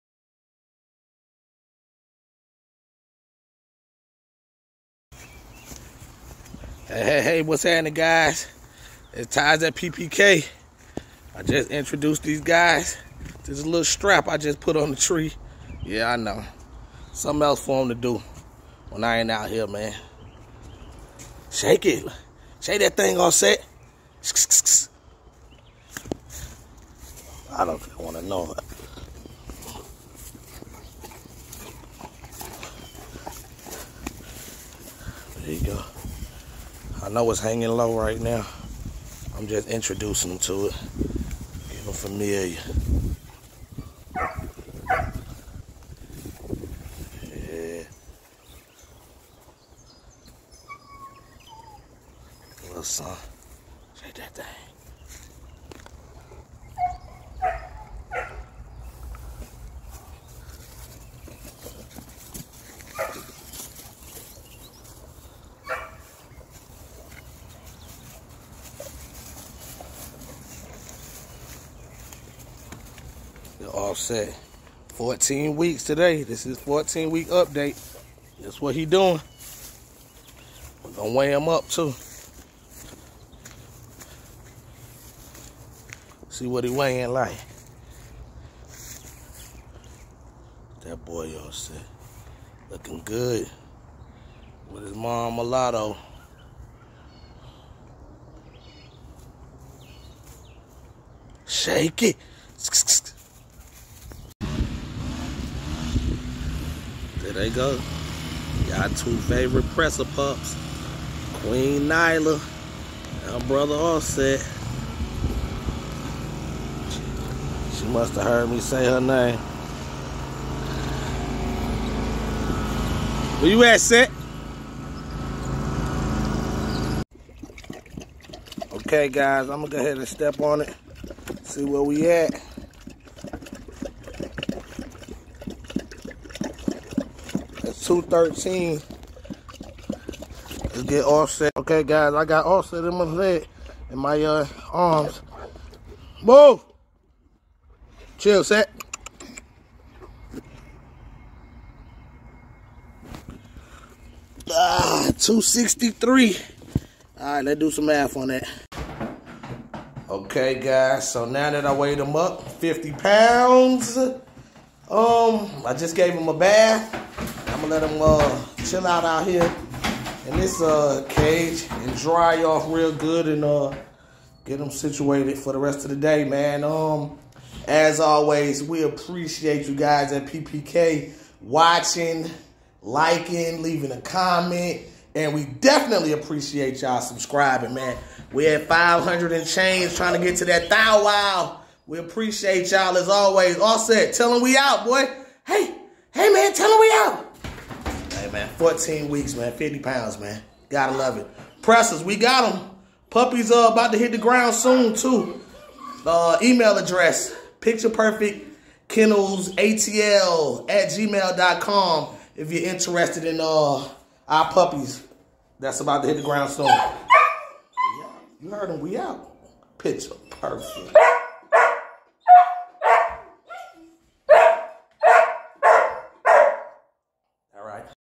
Presa Canario Puppy 👑OFFSET 🐶 14 Week Update!
Your Favorite Presa Canario Puppy 👑OFFSET is Growing Fast. 14 Weeks & 50 lbs. In this Video he is having Some Playtime with his Mother MuLotto and Sister Nyla.